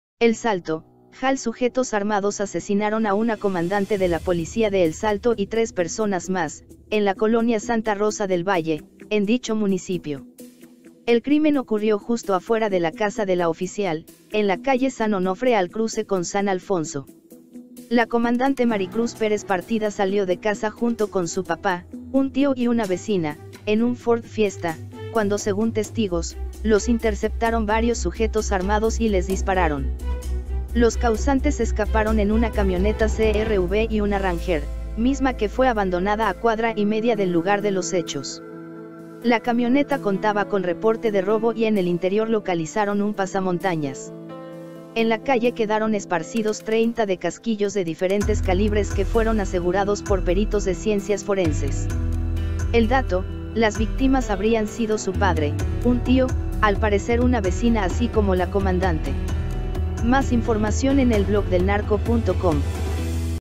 Ejecutan a mujer comandante de Policía de El Salto, Jalisco y a su familia
El Salto, Jal.- Sujetos armados asesinaron a una comandante de la Policía de El Salto y tres personas más, en la colonia Santa Rosa del Valle, en dicho municipio.

El crimen ocurrió justo afuera de la casa de la oficial, en la calle San Onofre al cruce con San Alfonso.  La comandante Maricruz Pérez Partida salió de casa junto con su papá, un tío y una vecina, en un Ford Fiesta, cuando según testigos, los interceptaron varios sujetos armados y les dispararon.